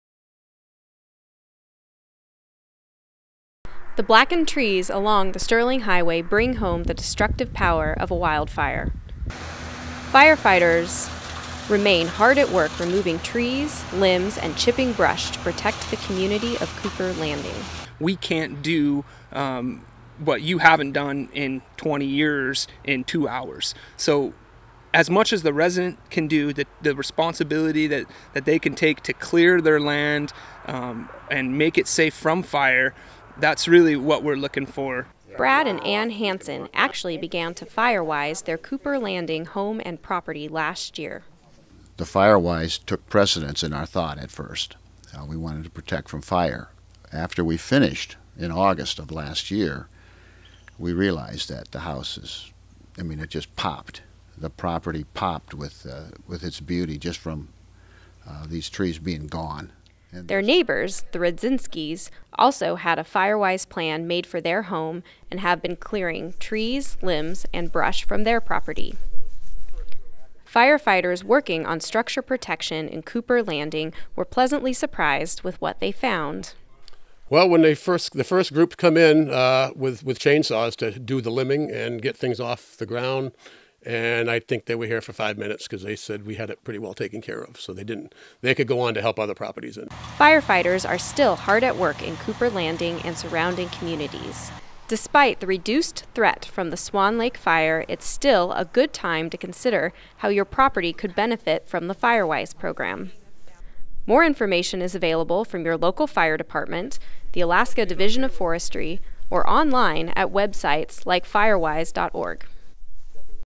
Protecting Cooper Landing Homes (Sept. 4, 2019)
The pilot cars are gone from the Sterling Highway, and the Evacuation Level for communities adjacent to the Swan Lake Fire have been downgraded. Residents around Cooper Landing have not let down their guard, and many realize fire danger is something that will not go away anytime soon.